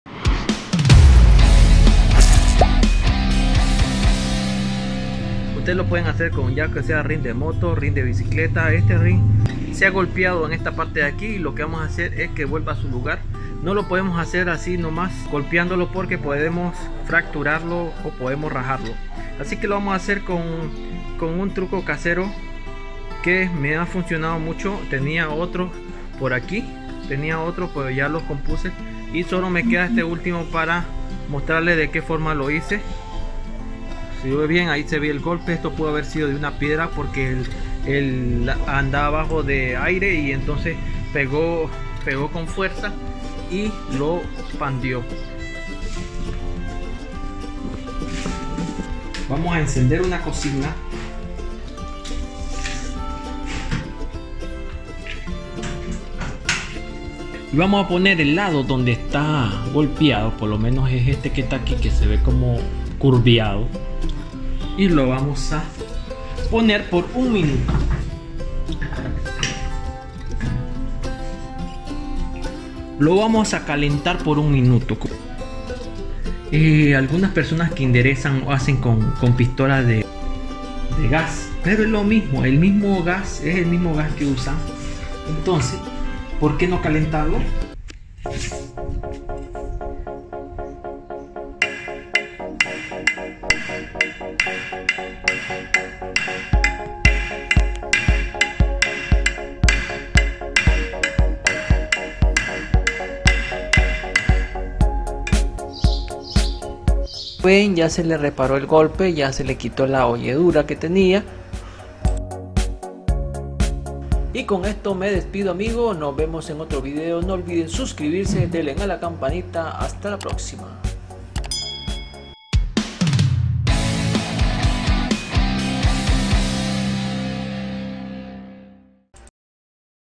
como reparar llantazo|bici|moto|monociclo electrico
Como quitar Golpes , abolladuras en rines de aluminio , rines de bicicleta , moto, rines de monociclo eléctrico. con un método ensillo y rápido de hacer.